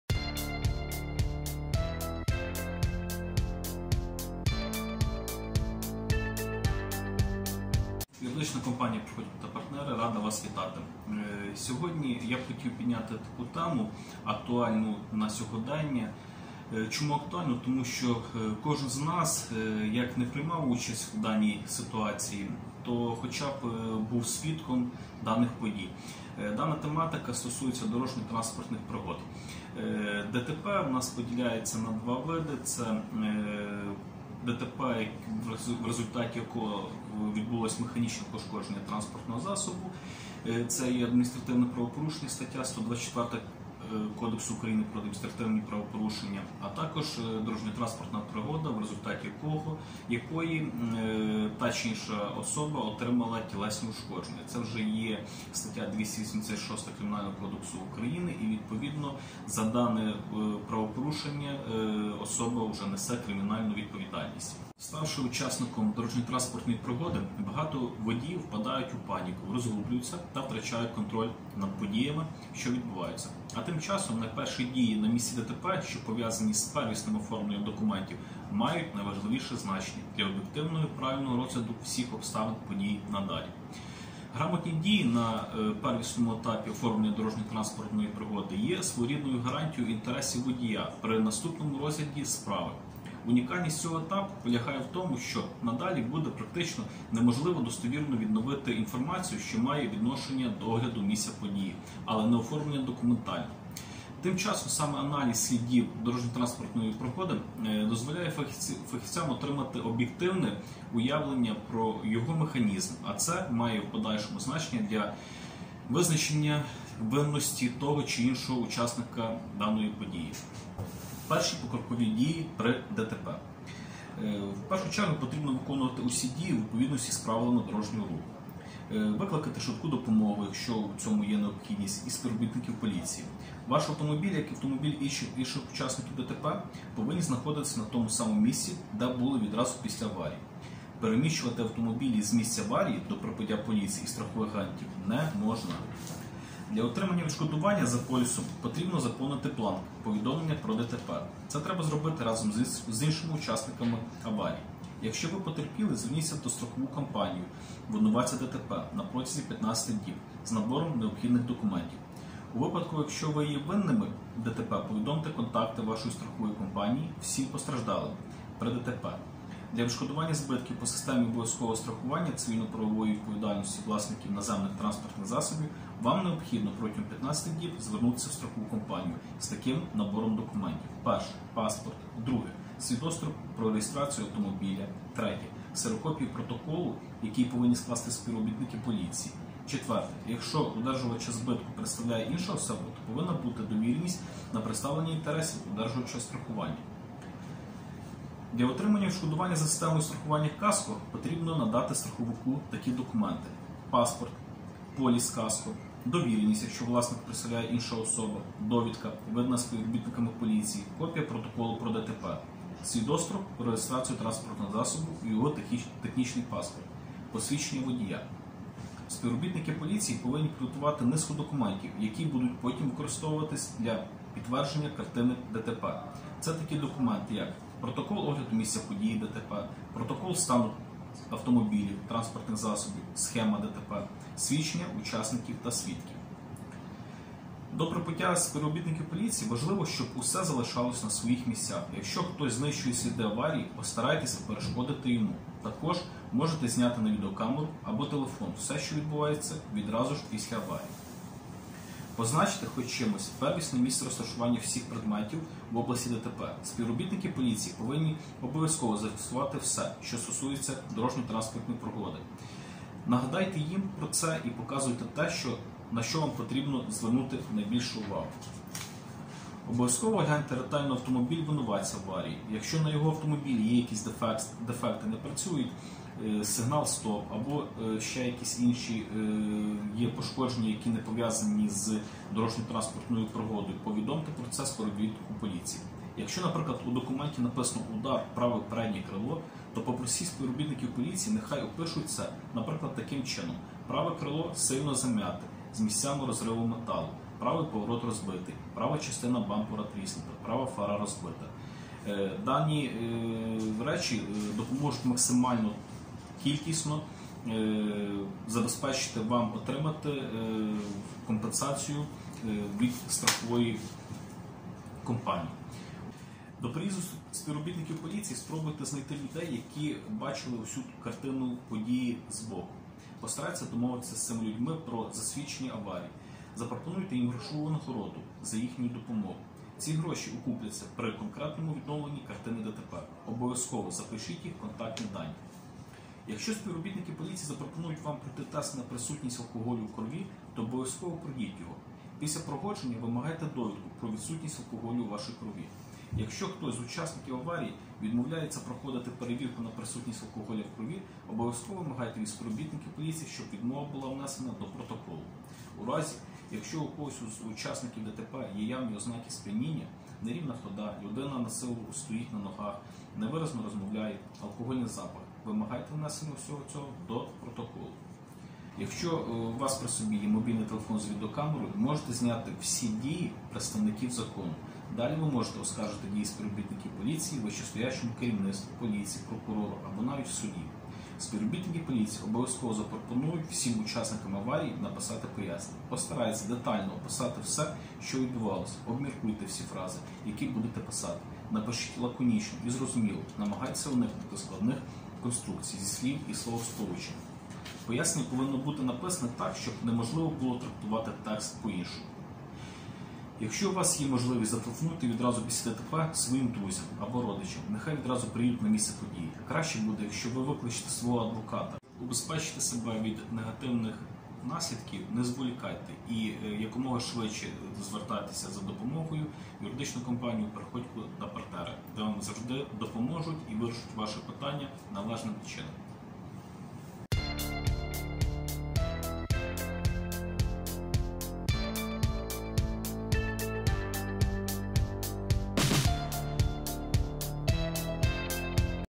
ПОТРАПИЛИ В ДТП? ЩО РОБИТИ? ПОРАДИ АДВОКАТА (2019)
Грамотні дії на первісному етапі оформлення дорожньо-транспортного пригоди є своєрідною гарантією інтересів водія при наступному розгляді справи. Унікальність цього етапу полягає в тому, що надалі буде практично неможливо достовірно відновити інформацію, що має відношення до огляду місця події, але не оформлена документально.
Співробітники поліції, що прибули на місце події, зобов’язані не тільки опитати всіх учасників та очевидців, але й скласти протокол огляду місця події та прикласти до нього схему ДТП, а також протоколи огляду транспортних засобів, із зазначенням отриманих ними технічних ушкоджень. В цій ситуації існує декілька підводних каменів.

Перші покрокові дії при ДТП
Виконати усі дії у відповідності з Правилами Дорожнього Руху.
Викликати швидку допомогу (якщо треба) i співробітників поліції. Ваш автомобіль, як i автомобілі інших учасників ДТП повинні знаходиться на тому самому місці, де були вiдразу пiсля аварiї. Перемiщувати автомобiлi з мiсця аварiї до прибуття полiцiї i страхових агентiв не можна.
Для отримання вiдшкодування за полiсом потрiбно заповнити бланк повiдомлення про ДТП, це треба зробити разом з iншими учасниками аварiї.
Для отримання вiдшкодування за системою страхування КАСКО потрiбно надати страховику такi документи:
Паспорт.
Полiс КАСКО.
Довiренiсть (якщо власника представляє iнша особа).
Довiдка, видана спiвробiтниками полiцiї.
Копiя протоколу про ДТП.
Свiдоцтво про реєстрацiю транспортного засобу i його технiчний паспорт.
Посвiдчення водiя.
Спiвробiтники полiцiї повиннi пiдготувати низку документiв, якi будуть потiм використовуватися для вiдтворення картини ДТП. А саме:
Протокол про огляд мiсця ДТП.
Протокол стану автомобiлiв(транспортних засобiв).
Схема ДТП.
Свiдчення учасникiв i свiдкiв.
До прибуття спiвробiтникiв полiцiї по можливості робіть фото та відео фіксацію подій, що відбуваються на місці аварії.
Позначте хоч чимось первiсне мiсце розташування всiх предметiв в областi ДТП. Спiвробiтники полiцiї повиннi обов’язково зафiксувати все, що стосується ДТП, нагадуйте їм про це i показуйте те, на що потрiбно звернути увагу.
Обов’язково огляньте ретельно автомобiль винуватця аварiї, якщо на його автомобiлi є якiсь дефекти (не працюють стоп-сигнали або ще щось, що може бути причиною ДТП), повiдомте про це спiвробiтникiв полiцiї. Якщо, наприклад, у документi написано “удар в праве переднє крило”, попросіть спiвробiтникiв полiцiї нехай опишуть це, наприклад, так: “праве крило сильно зiм’яте з мiсцями розриву металу, правий поворот розбитий, права частина бампера трiснута, права фара розбита, правий захисний чохол пiд крилом розбитий, права стiйка пошкоджена”. Максимальна кiлькiсть iнформацiї дасть вам можливiсть отримати максимальну компенсацiю.
У вище описаних прикладах ви могли отримати два рiзнi розмiри компенсацiї, в першому випадку ви отримали б компенсацiю на замiну крила, у iншому випадку вам проведуть серйозний ремонт автомобiля iз замiною багатьох запчастин. Обов’язково перечитайте всi складенi спiвробiтниками полiцiї документи. Якщо вас щось не влаштовує, напишiть свої зауваження.
До приїзду спiвробiтникiв полiцiї спробуйте знайти людей, якi бачили усю картину подiї з боку. Обов’язково запишiть їх контактнi данi.
Якщо спiвробiтники полiцiї запропонують вам пройти тест на присутнiсть алкоголю в кровi, то обов’язково пройдiть його. Пiсля проходження вимагайте довiдку про вiдсутнiсть алкоголю у вашiй кровi. Якщо хтось з учасникiв аварiї вiдмовляється проходити перевiрку на присутнiсть алкоголю в кровi, обов’язково вимагайте вiд спiвробiтникiв полiцiї, щоб вiдмова була внесена до протоколу. У разi якщо у когось з учасникiв ДТП є явнi ознаки сп’янiння (нерiвна хода, людина насилу стоїть на ногах, невиразно розмовляє, алкогольний запах), вимагайте внесення усього цього до протоколу.
У разi, якщо вам не подобаються дiї спiвробiтникiв полiцiї, постарайтеся не вступати з ними в конфлiкт. Усе що вас не влаштовує, ви можете внести у вiдповiднi документи i вирiшити проблему бiльш спокiйно. Також якщо спiвробiтники полiцiї грубо порушують закон, намагайтеся бути стриманим i внесiть всi свої зауваження до протоколу ДТП.
Якщо у вас є мобiльний телефон з вiдеокамерою можете зняти всi дiї представникiв закону. Спiвробiтники полiцiї обов’язково запропонують всiм учасникам аварiї написати пояснення. Постарайтеся детально описати все, що вiдбулося, обмiркуйте всi фрази, якi будете писати. Пишiть лаконiчно i зрозумiло, намагайтеся уникати складних конструкцiй iз слiв i словосполучень. Пояснення повинне бути написане так, щоб неможливо було трактувати текст по-iншому.
Якщо у вас є можливiсть, зателефонуйте вiдразу пiсля ДТП своїм друзям або родичам, нехай вiдразу приїдуть на мiсце подiї. Краще буде, якщо ви викличете свого адвоката.